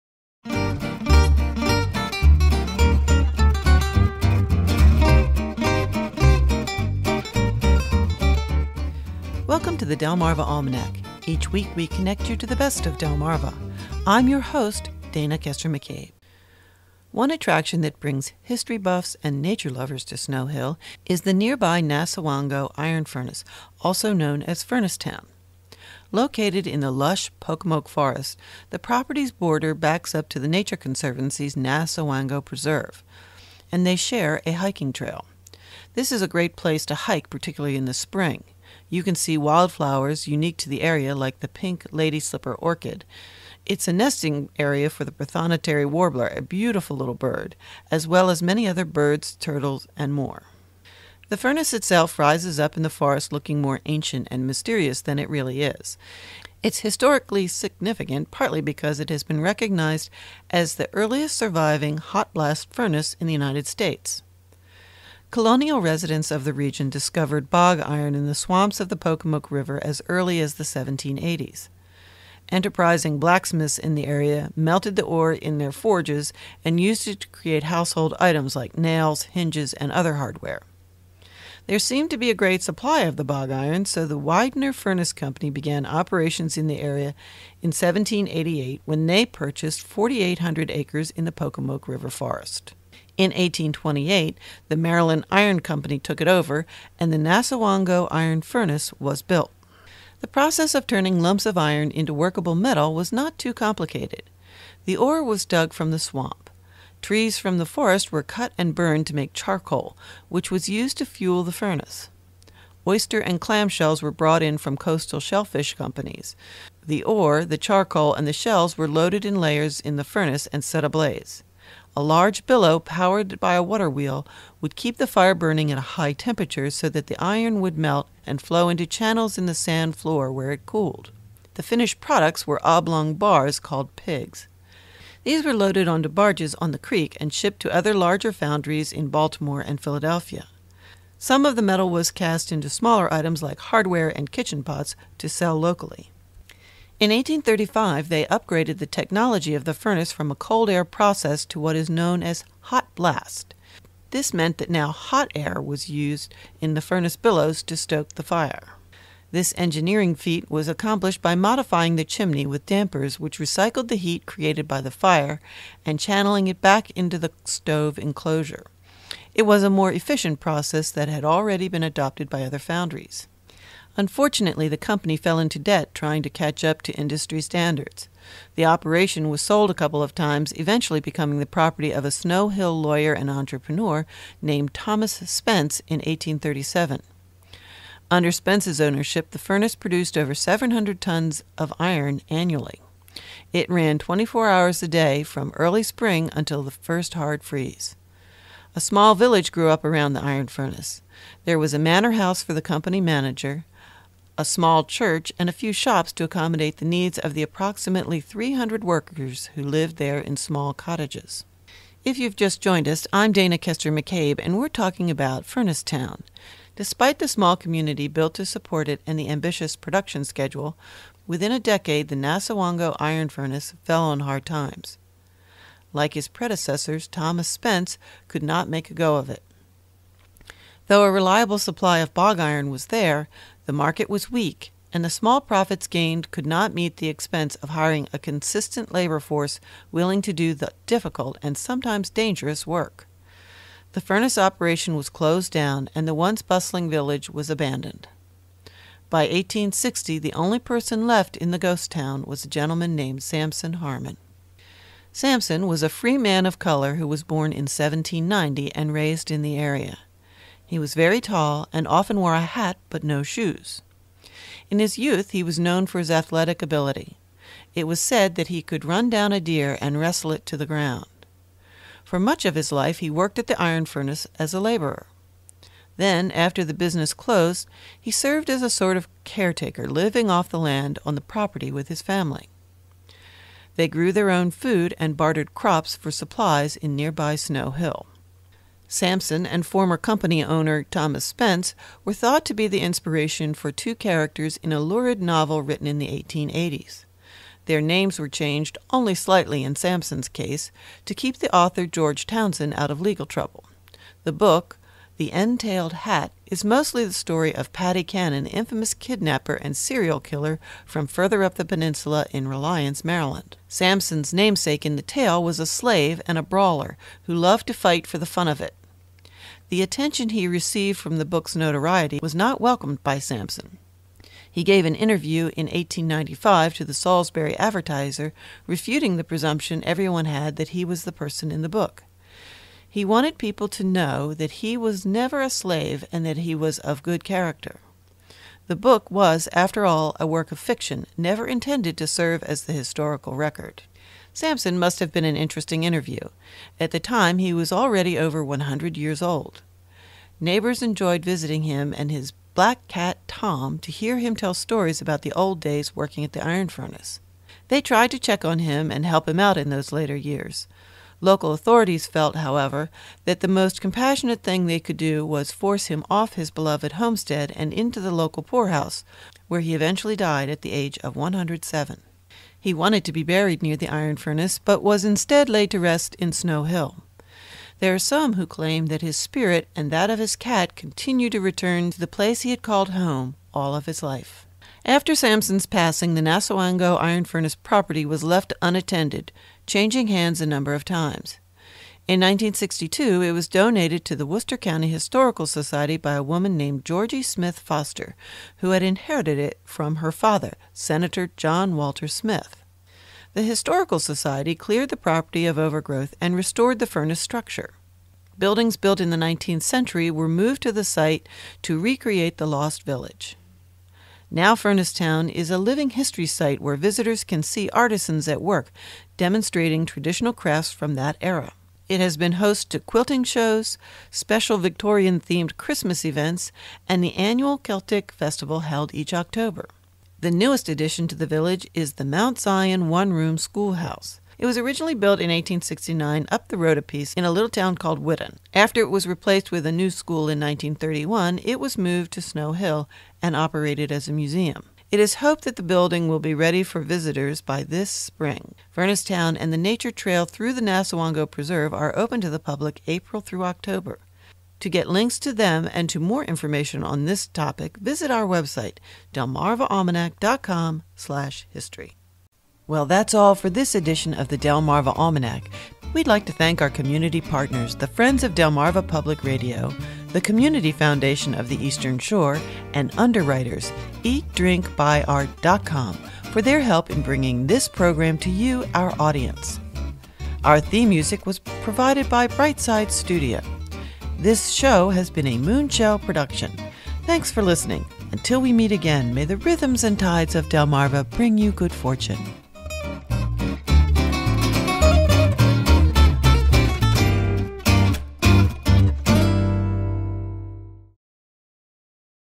The Nassawango Iron Furnace
The Nassawango Iron Furnace rises up in the forest looking ancient and mysterious. It is an historic reminder of the beginnings of the industrial revolution.